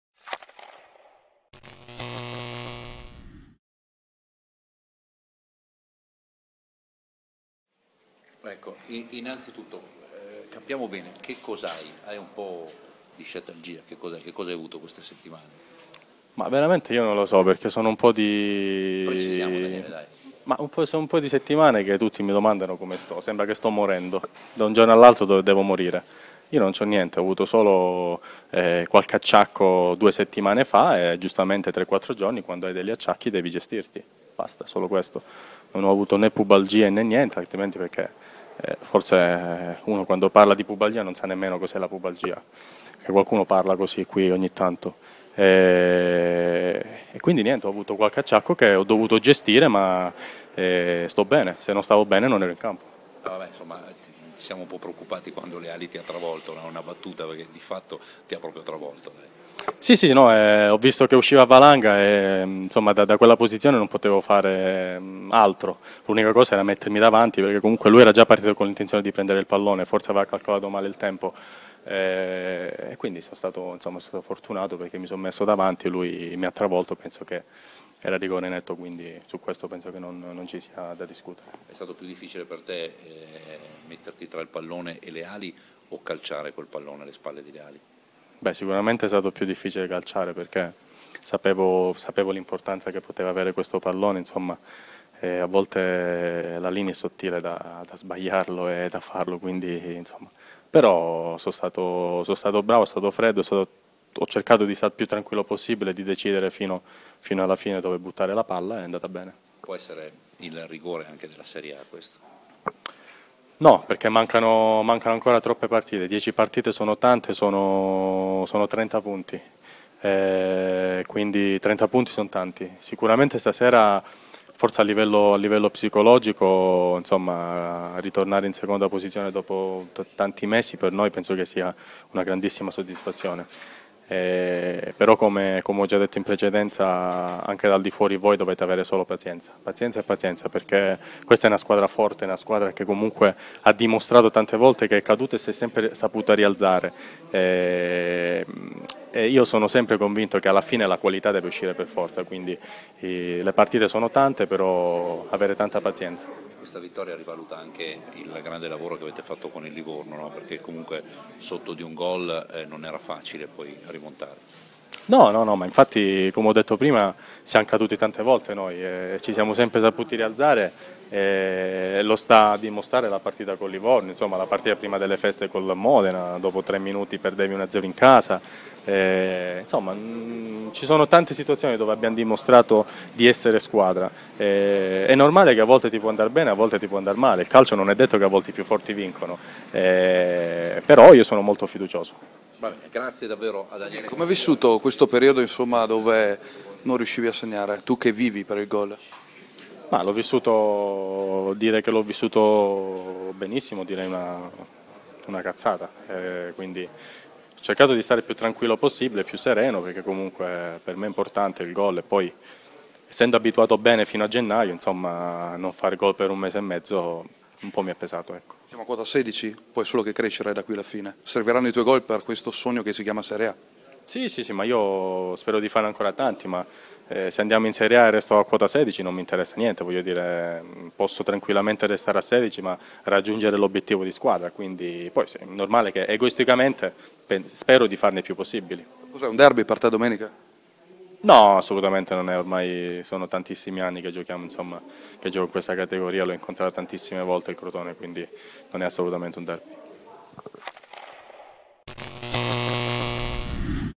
Il ritorno del bomber
Daniele Cacia dopo un mese e mezzo si è sbloccato regalando dagli undici metri la vittoria ai gialloblù a Lanciano